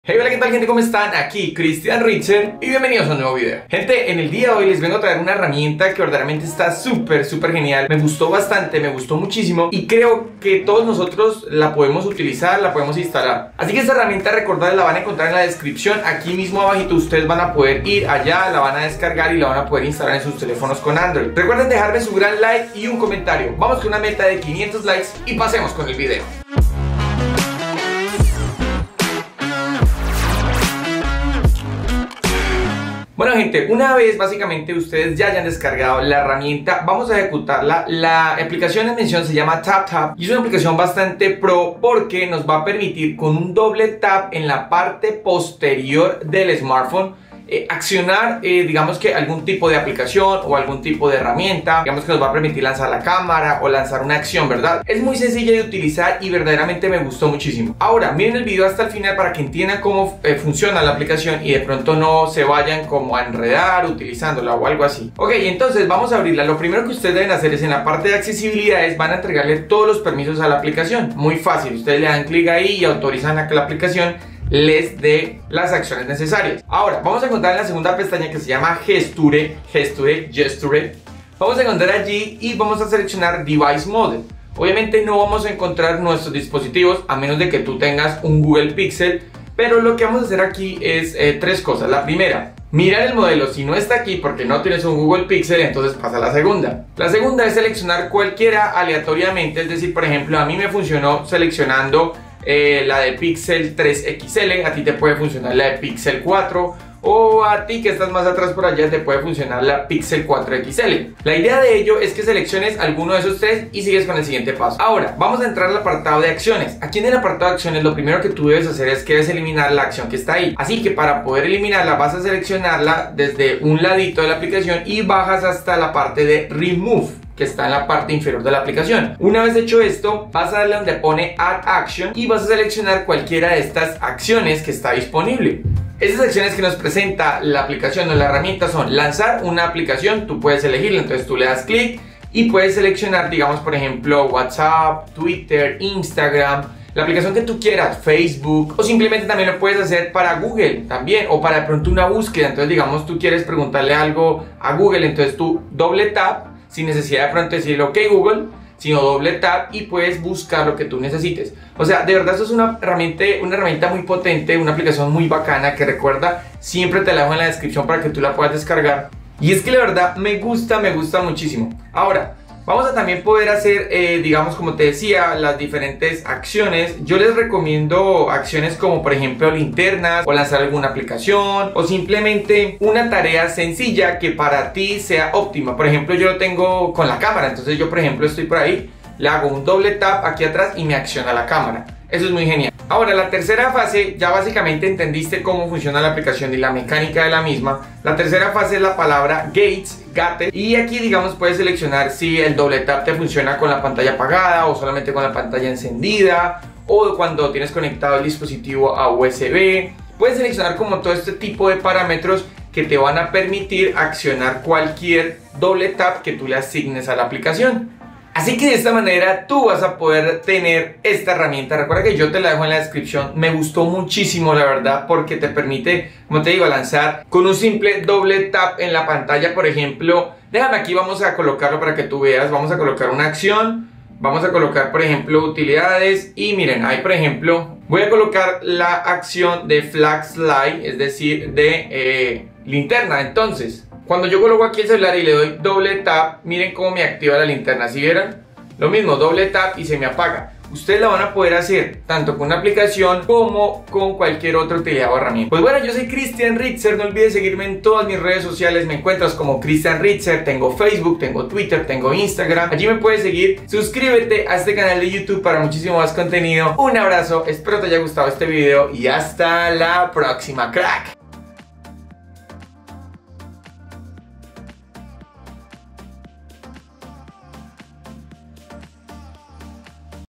ACTIVA EL SENSOR SECRETO EN CUALQUIER ANDROID 2020
ACTIVA EL SENSOR SECRETO DE LA PARTE DE ATRAS DE TU ANDROID

Aqui te dejo un video donde te enseño a como activar el sensor trasero de tu telefono con android para que puedas lanzar cualquier aplicacion de una forma rapiida.